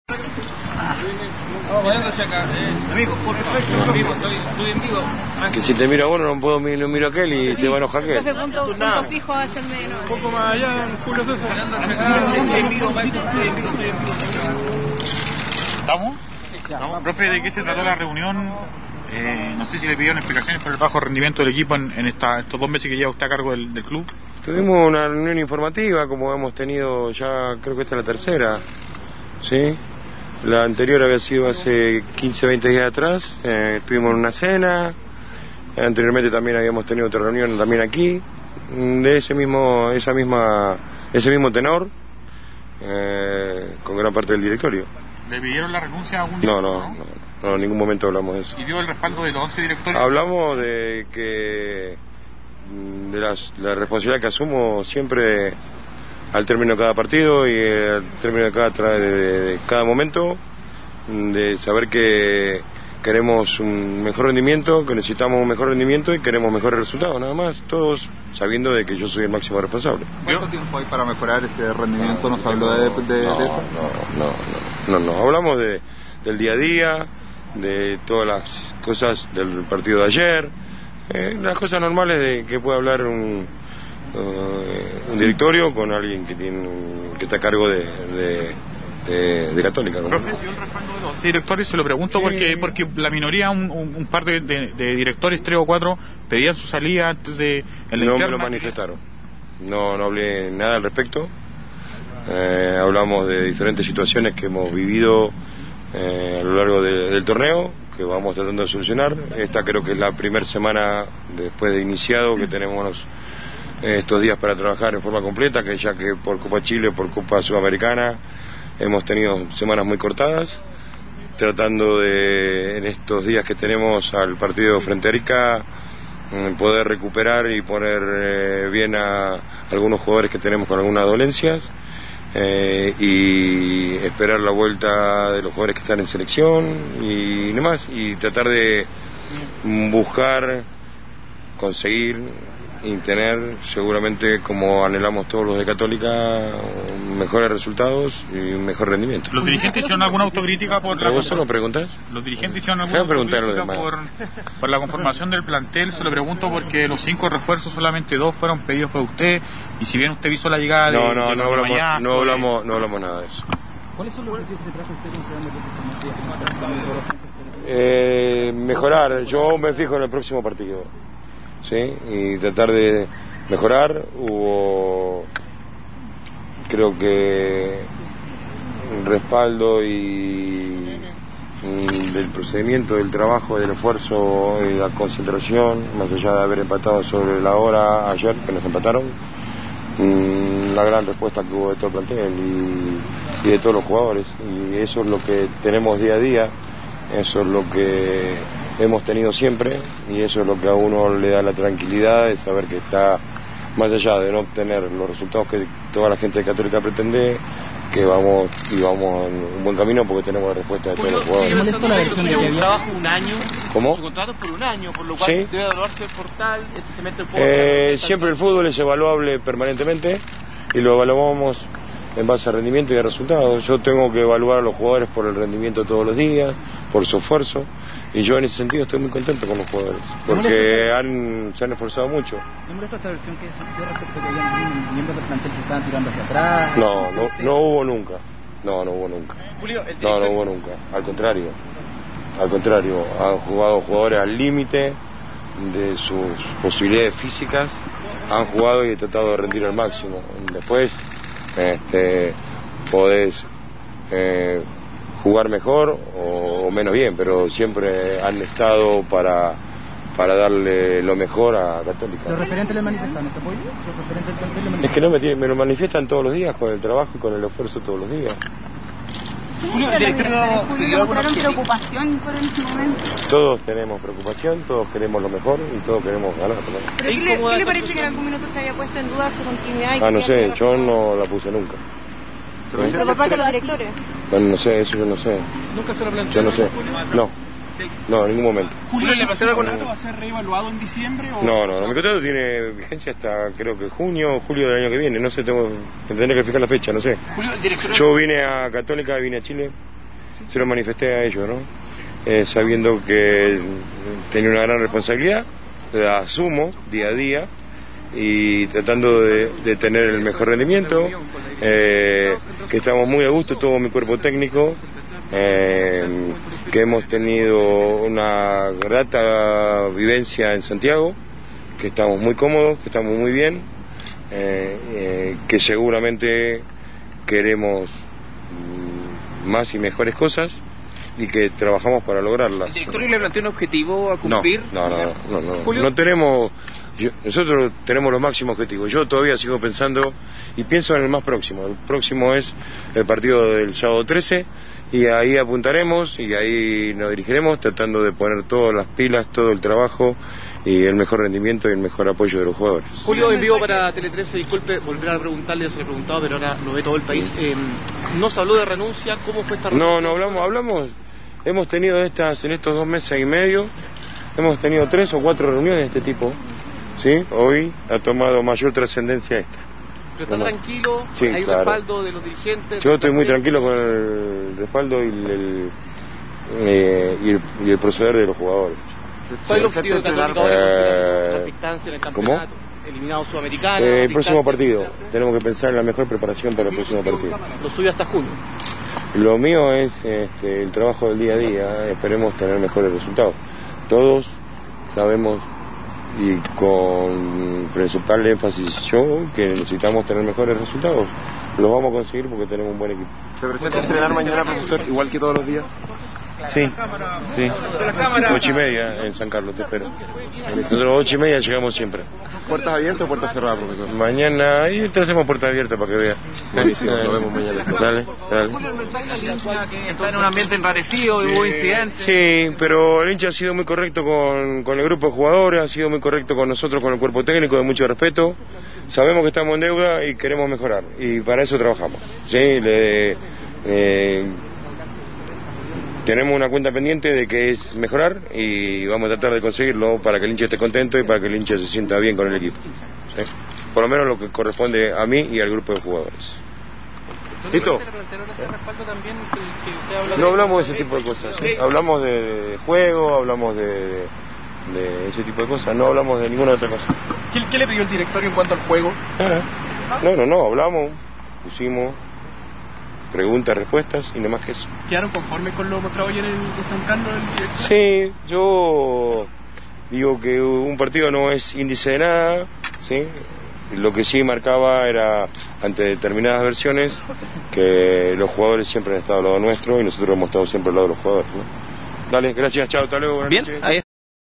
Falcioni seguirá en la banca de Universidad Católica
El conjunto de la franja se mantiene con un mal rendimiento en la cancha, sin poder ganar en las últimas cinco fechas.